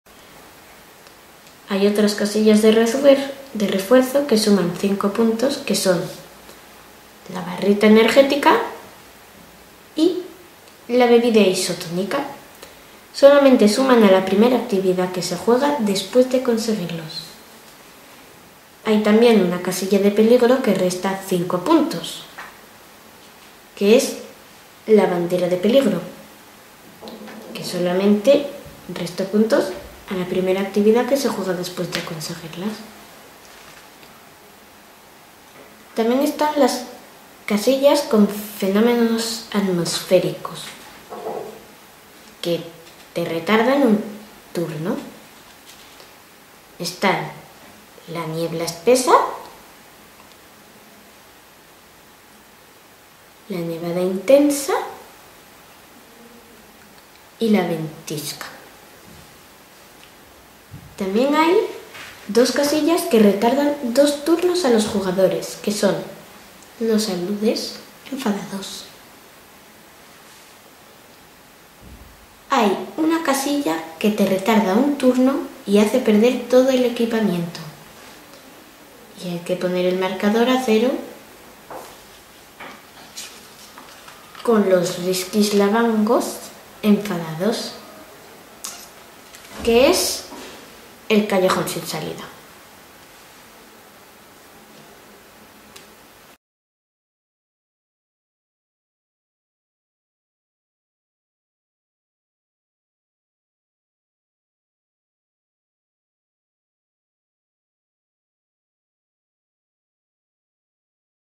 Casillas - Como jugar a Riski Lavango (2/7)
Conocer las casillas significativas de Riski Lavango